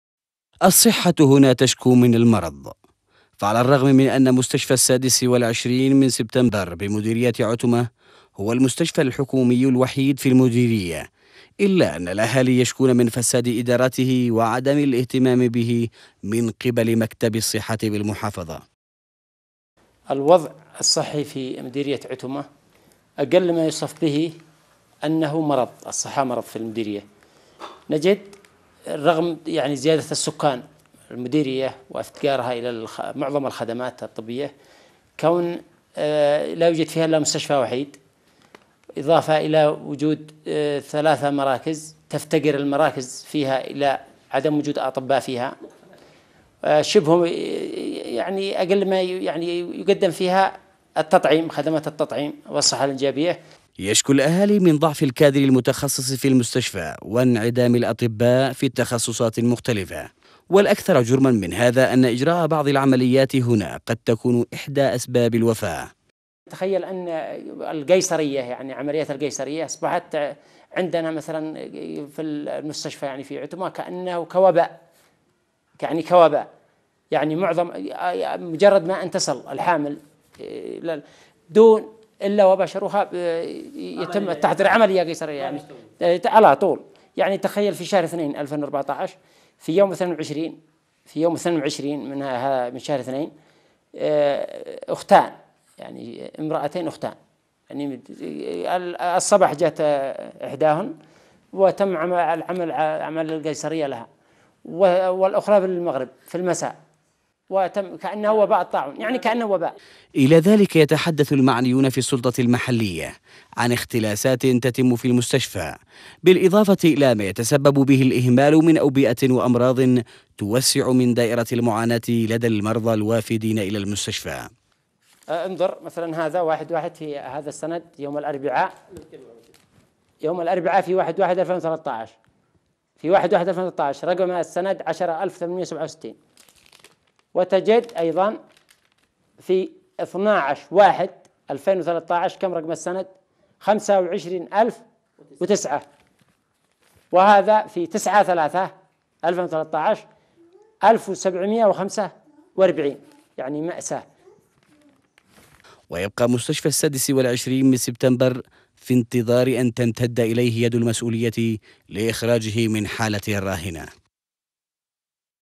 عتمة.. الصحة تعاني من وعكة الفساد
التردد الجديد للقناة على مدار النايلسات (11603 افقي H ) في مديرية عتمه فاتنة الجبال اليمنية تعاني الصحة من فساد ومشاكل تبدأ بعمليات اختلاس للمرضى في مستشفى سبتمبر وتنتهي بوفاة البعض نتيجة الأخطاء الطبية وضعف الكوادر المتخصصة.. نتابع
لمتابعة قناة سهيل على التردد التالي: 11595 عمودي
:زوروا موقعنا على الانترنت